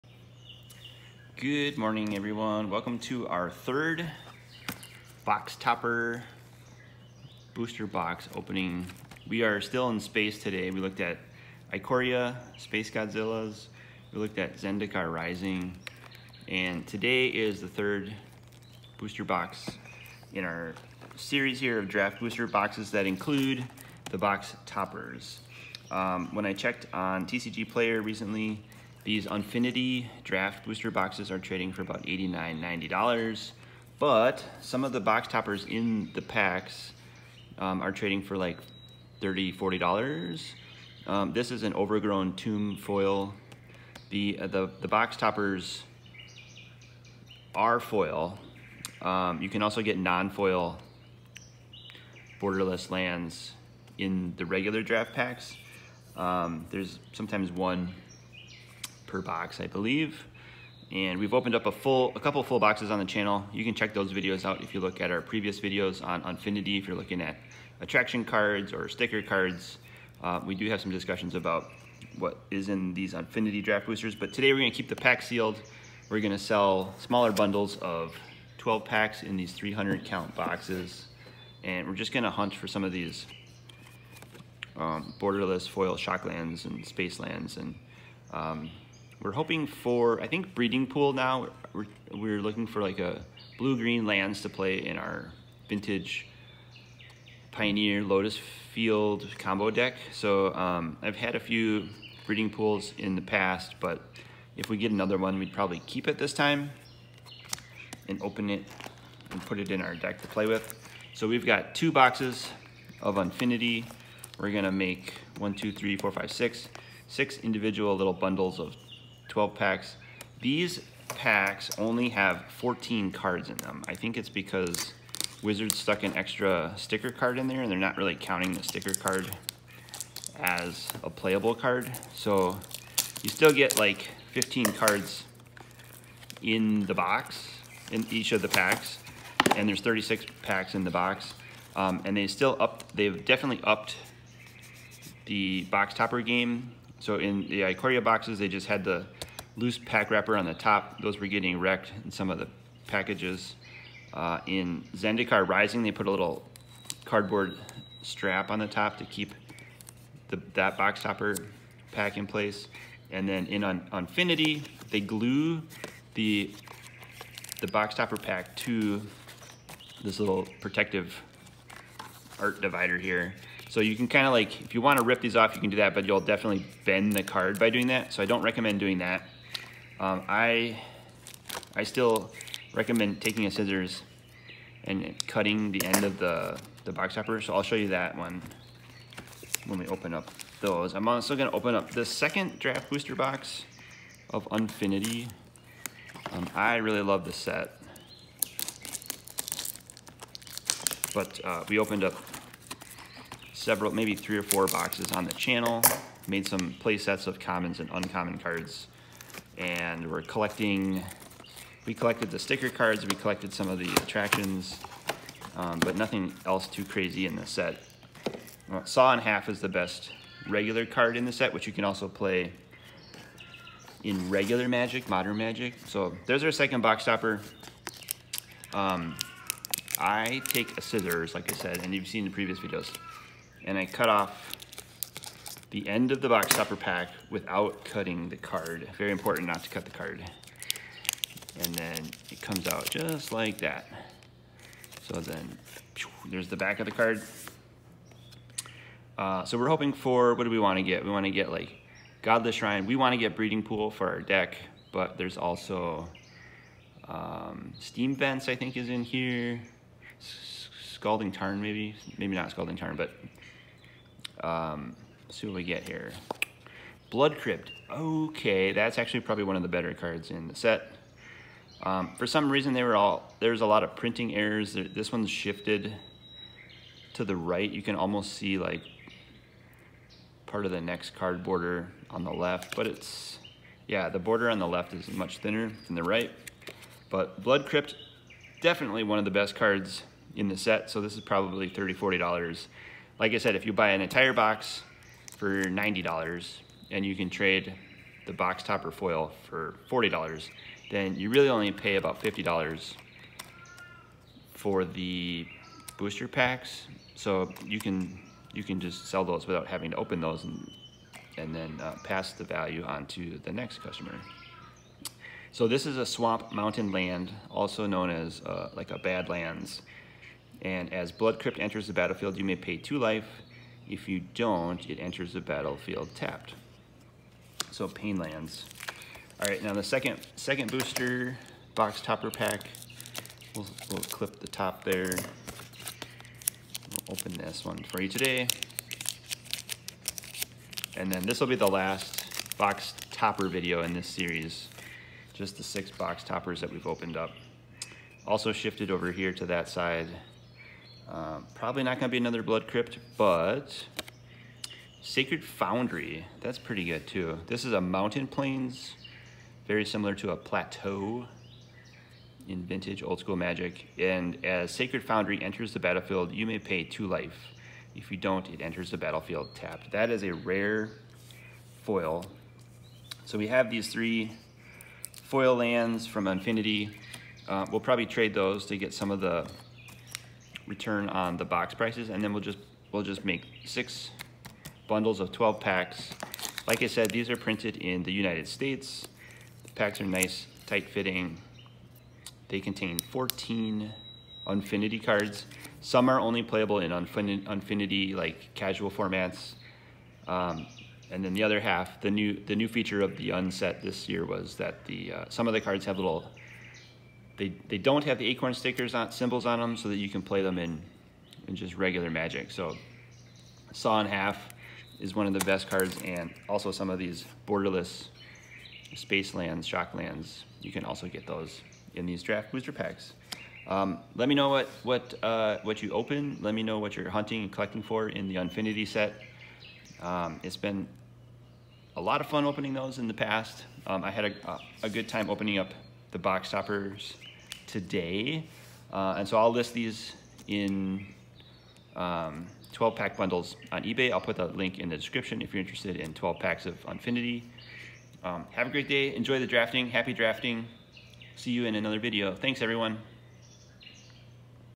Unfinity Box Topper Pack Openings!
Today we are opening two Magic the Gathering Unfinity Draft Booster Box Topper packs.